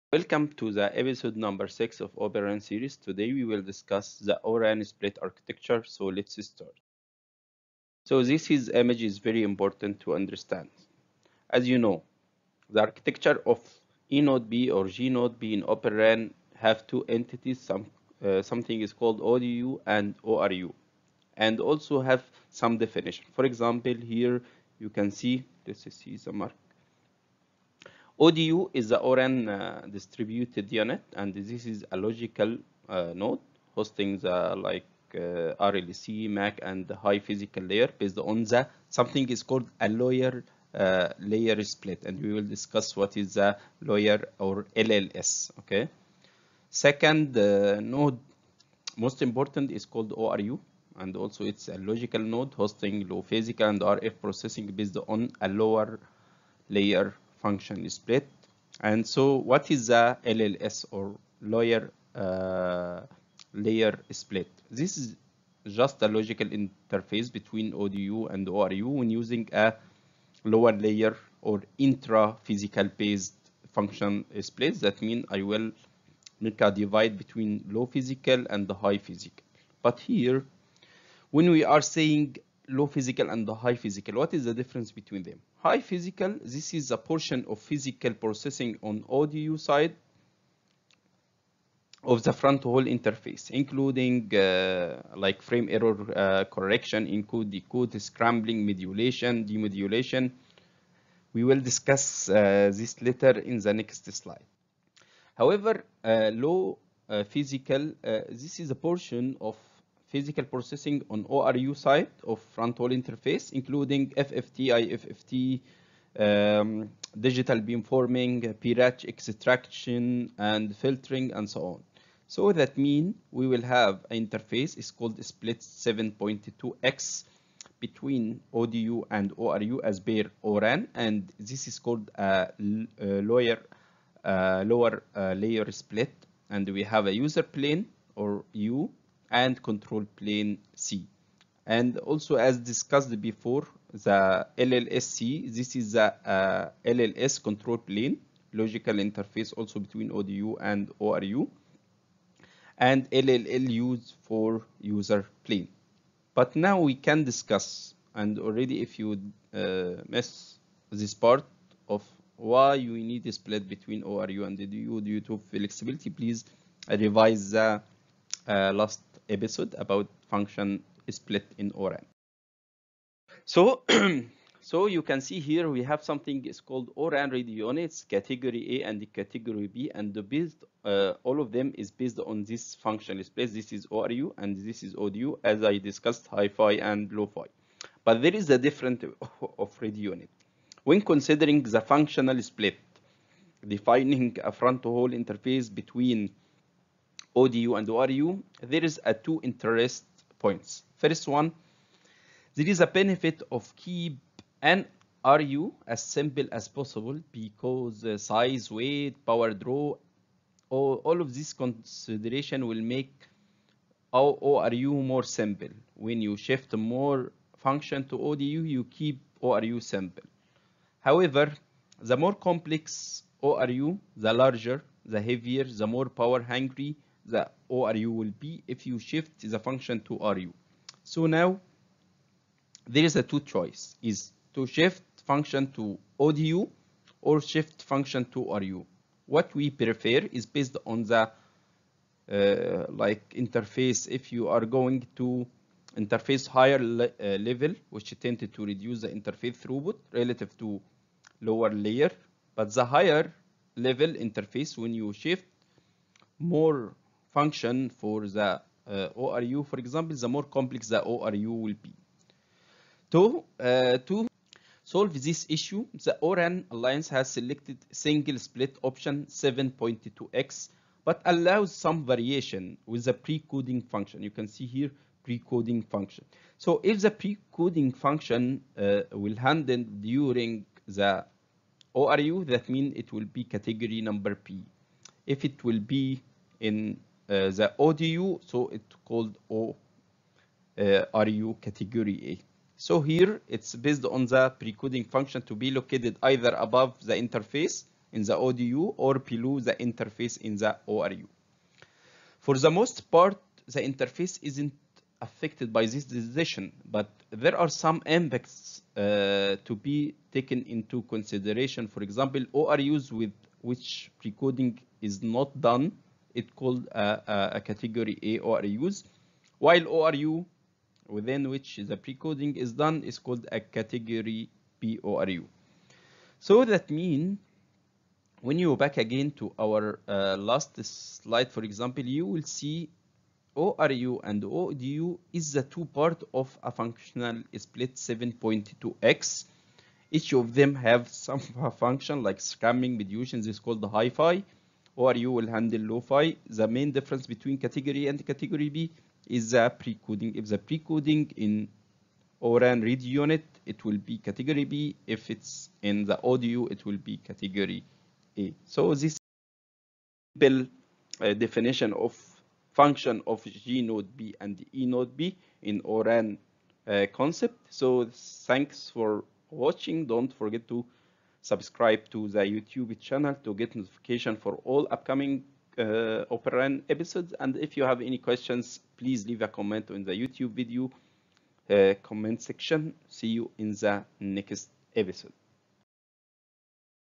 Open RAN Series l Episode 6 l Open RAN Split Architecture l English
🌎Open RAN Series🌎

🚦 Videos Series to explain the Open RAN Technology🚦

✔ Topics discussed in this session:
0:00 Introduction.
0:11 eNB/gNB Split Architecture.
2:54 ORAN Cat A/Cat B Radio Units (RU).